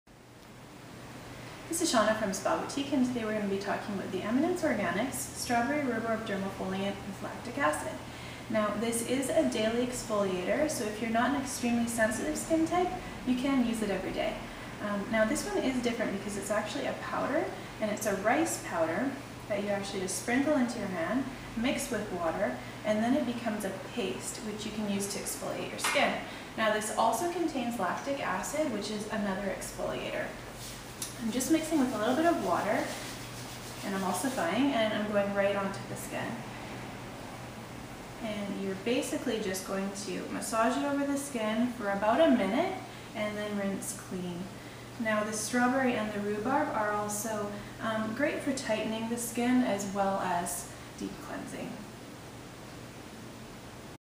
How to exfoliate with Eminence Strawberry Rhubarb Dermafoliant
Spa Boutique esthetician shows how to use this natural exfoliator.